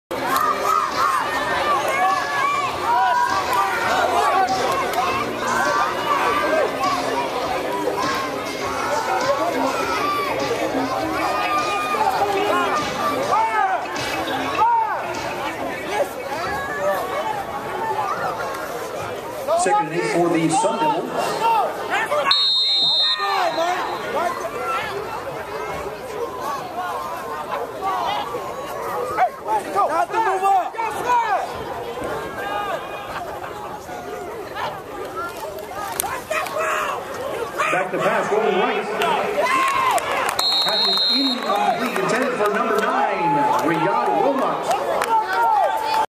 Okeechobee Wildcats Pass D 12U Orange Bowl 2014
The Okeechobee Wildcats pass defense against the North Miami Beach Sundevils during the 12U (135 pound) finals of the Orange Bowl Youth Football Alliance Championships, presented by Sports Authority at Alfonso Field at FIU Stadium in Miami. To qualify for the event, the Sundevils were Super Bowl champions of Miami Xtreme, and Okeechobee won the Glades Tri County League.